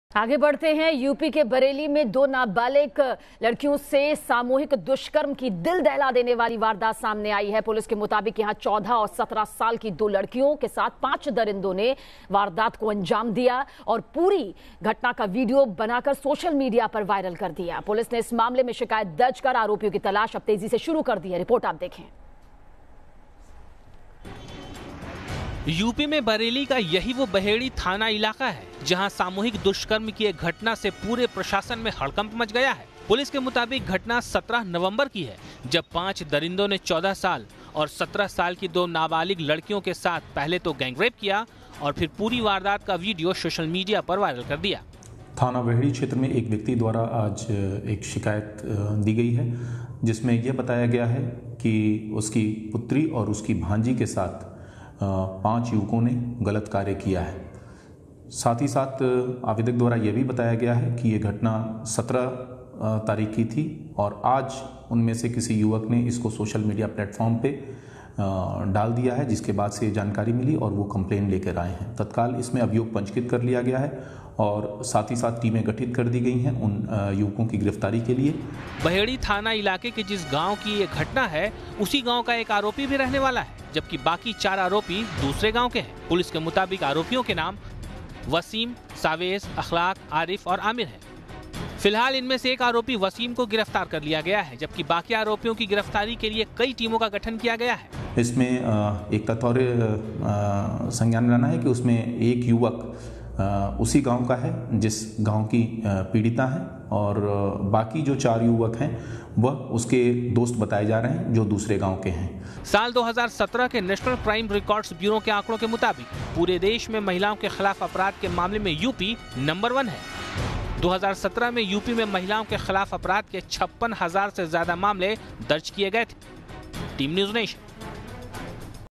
महिलाओं के लिए सुरक्षित नहीं यूपी, पुलिस दे रही कार्रवाई का आश्वासन
यूपी में महिला अब सुरक्षित महसूस नहीं कर रहीं और यूपी पुलिस केवल कार्रवाई का आश्वासन ही दे रही है. ताजा मामला उत्तर प्रदेश के बरेली का है जहां दो नाबालिगों के साथ हुआ दुष्कर्म. देखिए ये Video

News State Apps डाउनलोड करें:
Download Android and iOS Apps for more updates: